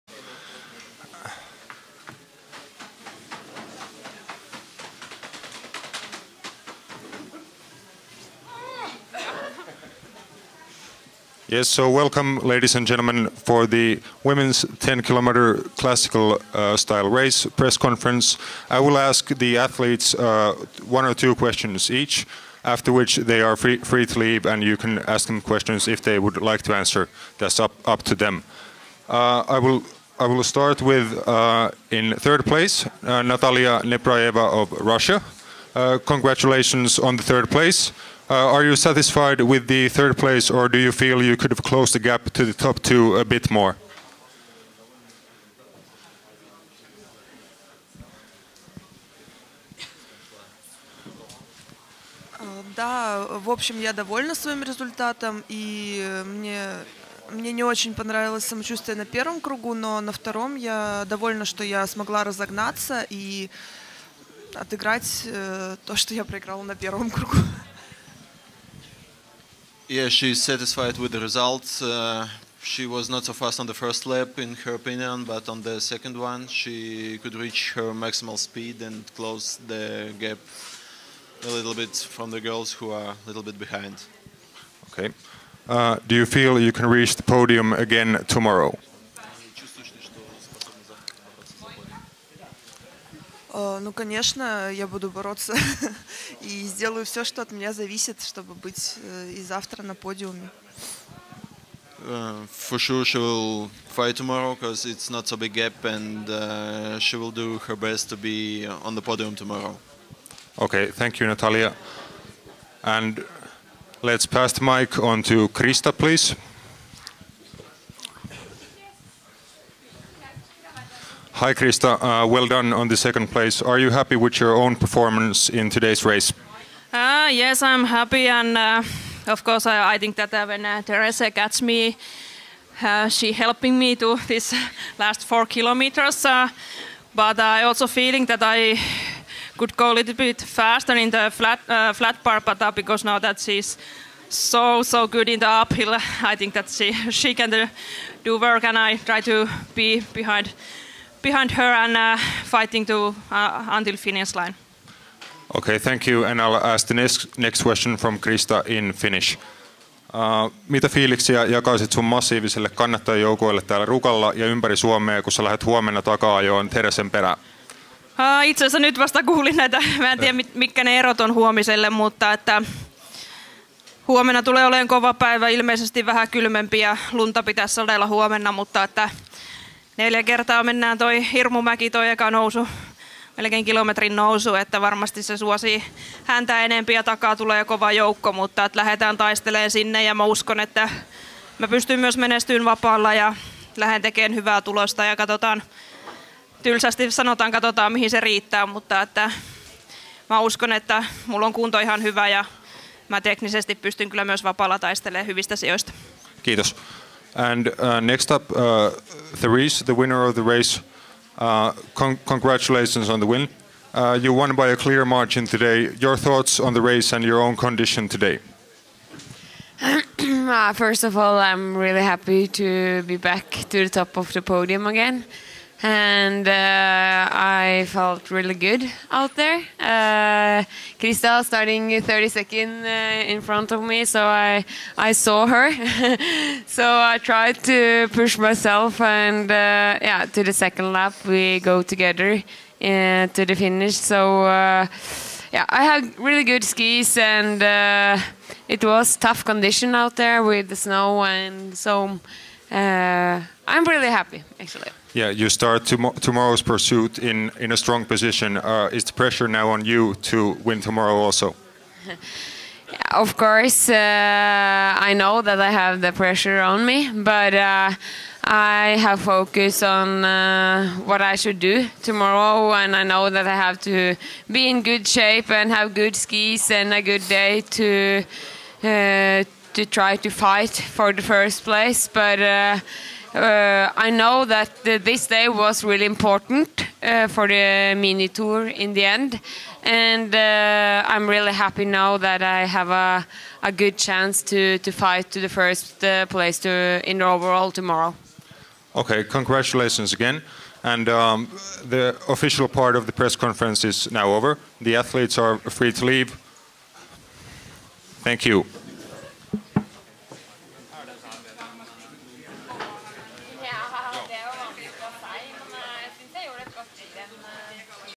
Press Conference 30.11.2019 / Cross-Country 10km Women Winners
Ruka Nordic Women 10km Cross-Country Winners Press